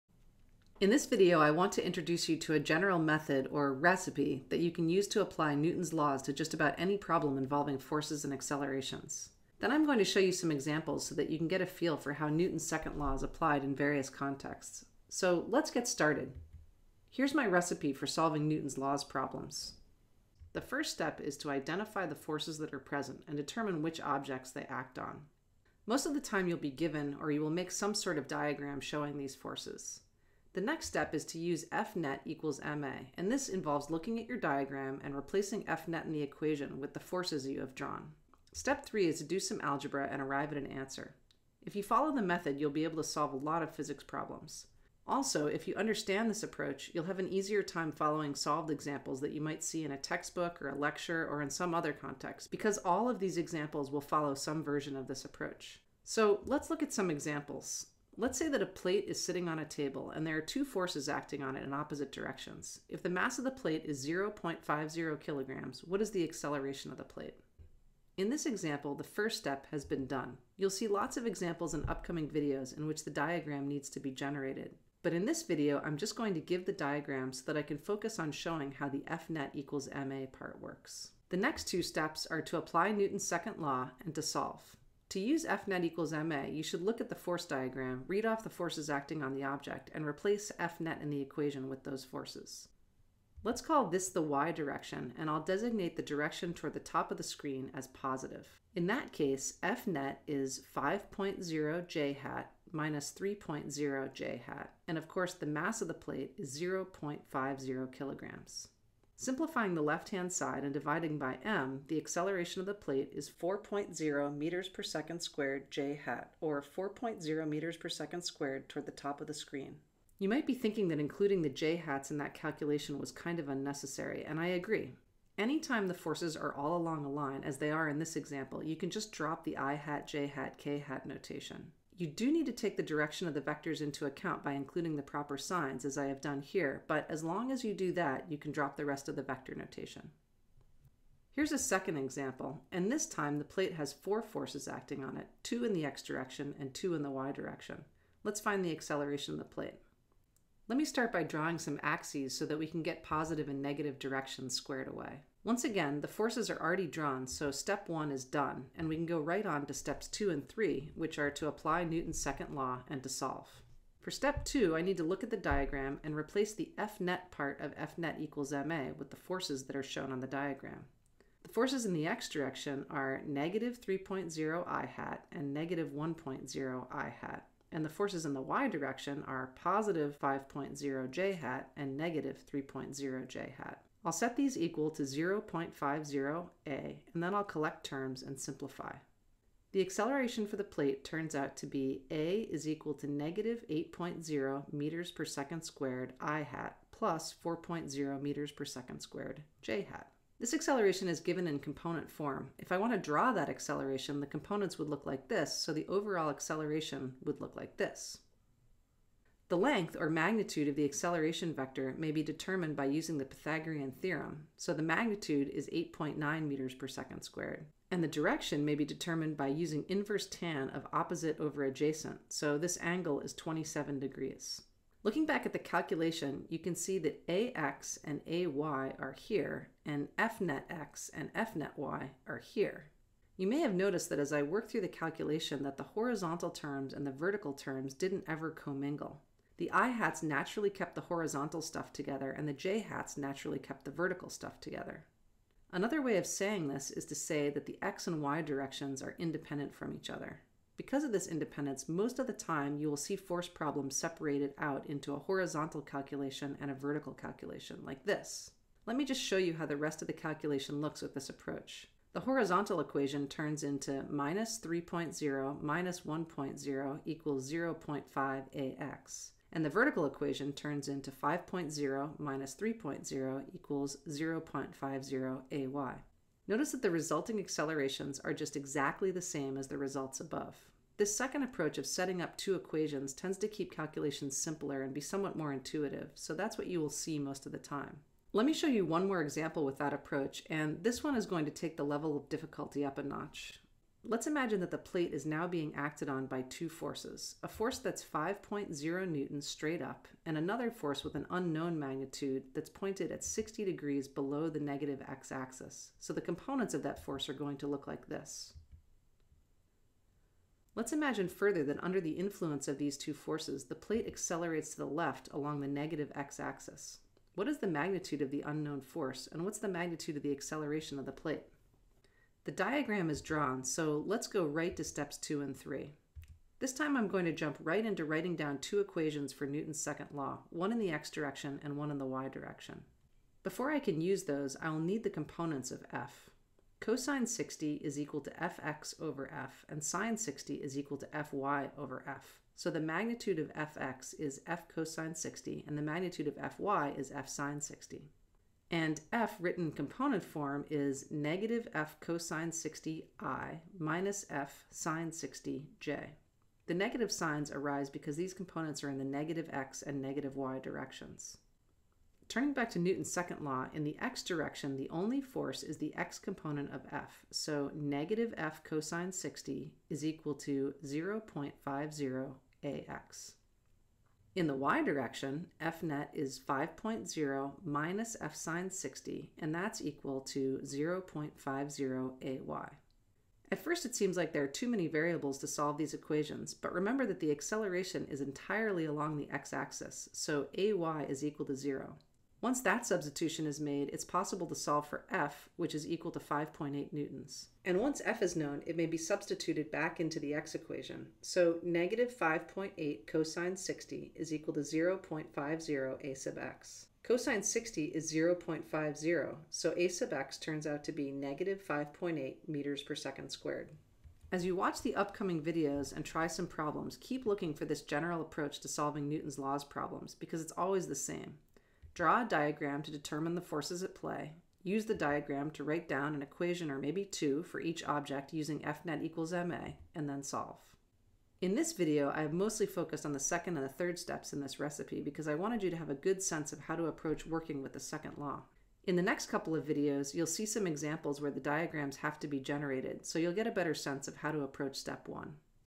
Applying Newton's Second Law
This video introduces a general method, or recipe, that may be used to apply Newton's laws to just about any problem involving forces and accelerations.  Several examples are included.